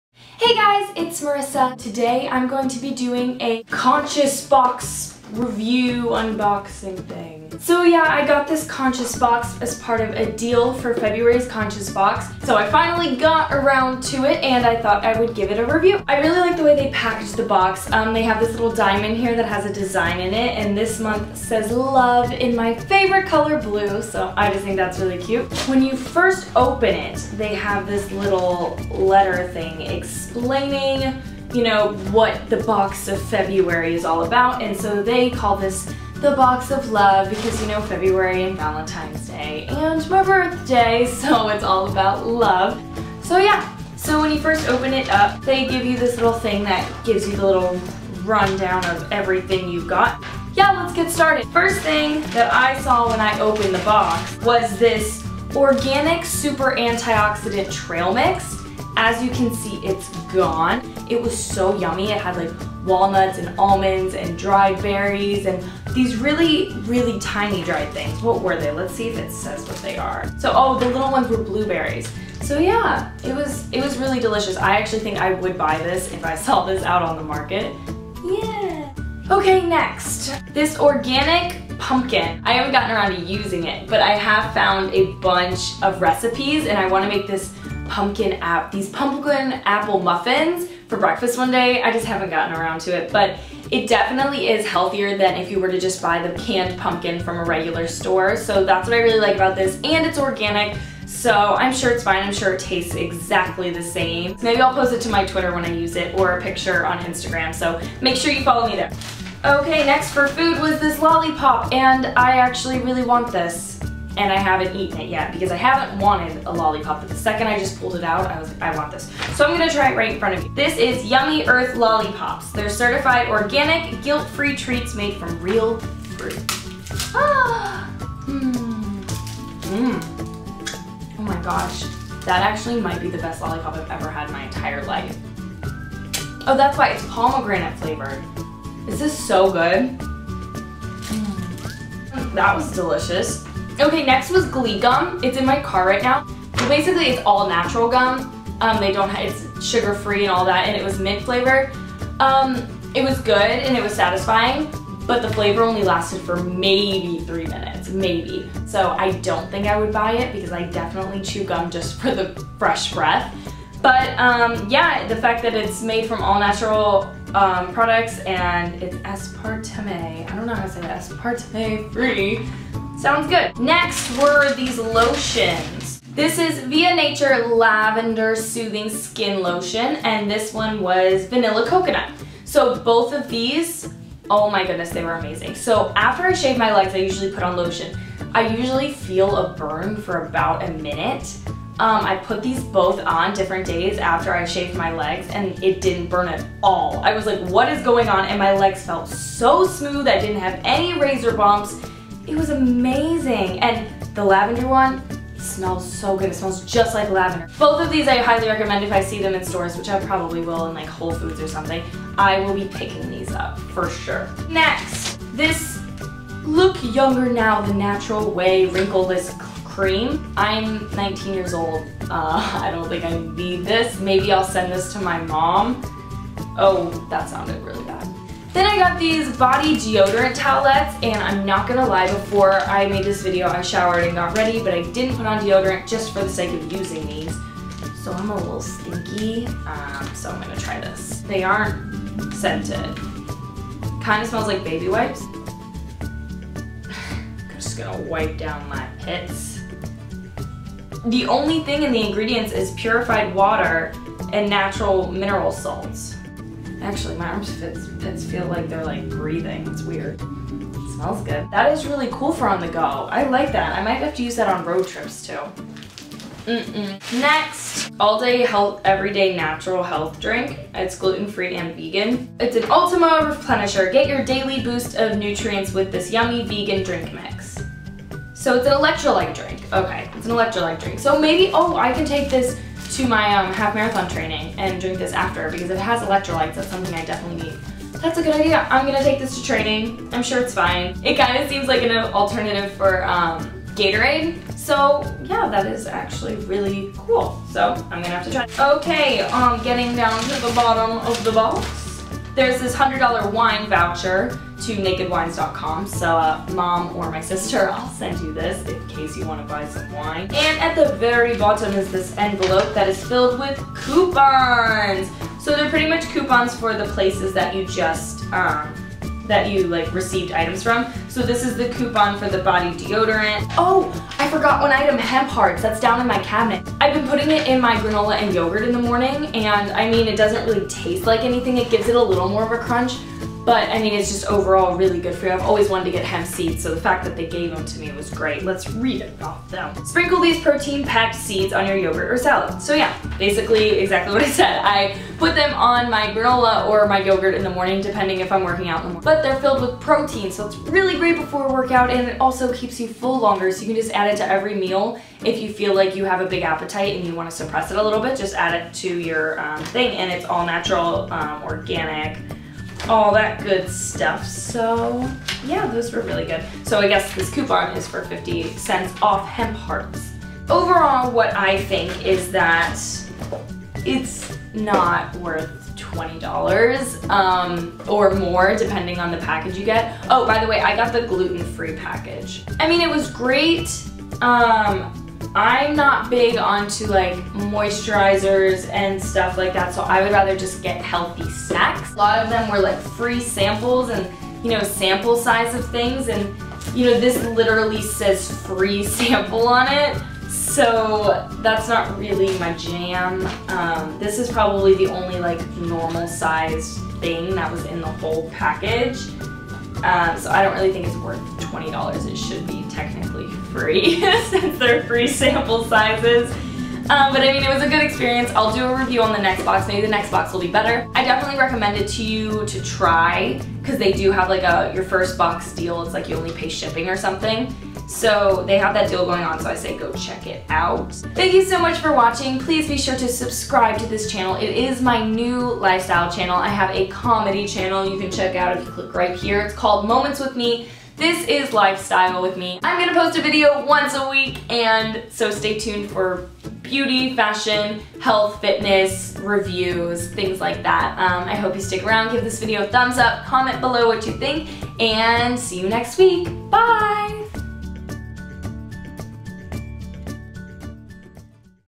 Conscious Box Review-Feb 2014
Here's my review of the February 2014 Gluten Free Conscious box

and get your first box!

Here are the items included:
-Via Nature Hydrating Lotion(Vanilla Coconut)
-Via Nature Soothing Lotion(Lavender)
-Aloevella Winkleless Cream
-Crystal Deoderant Towelettes
-Farmer's Market Foods Organic Pumpkin
-Glee Gum
-Manitoba Harvest Hemp Hearts
-Ultima Replenisher (I did try this at practice and it is SO yummy and kept me hydrated! yay!)
-Vital Choice Antioxidant Trail Mix
-Yummy Earth Lollipops